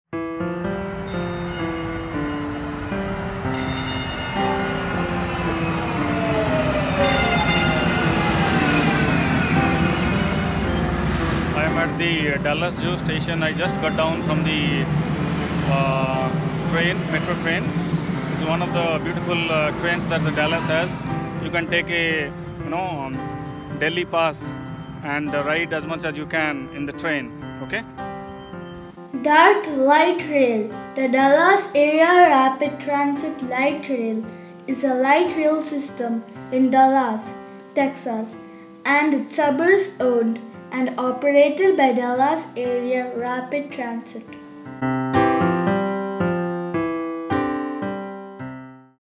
Dallas Area Rapid Transit Light Rail
DART Light Rail,    the Dallas Area Rapid Transit Light Rail,     is a light rail system in Dallas, Texas      and its suburbs owned     and operated by Dallas Area Rapid Transit.

The system comprises 93 miles among its four lines — the Red Line, the Blue Line, the Green Line, and the Orange Line.

It is the longest light rail system in the United States. As of the fourth quarter of 2017, the system has an average weekday ridership of 98,700, making it the largest light rail system in the Southern United States by ridership, and the sixth largest in the United States by ridership.

The system uses 163 light rail trains manufactured by Kinki Sharyo, with all trains being converted to "Super" LRVs which feature level boarding and higher passenger capacity.